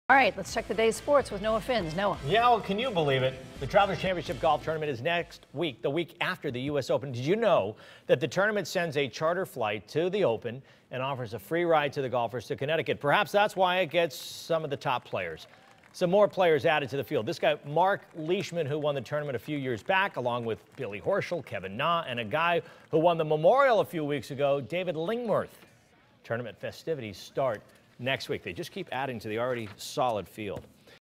Top players prepare for Travelers Championship next week
News 8's Noah Finz reports.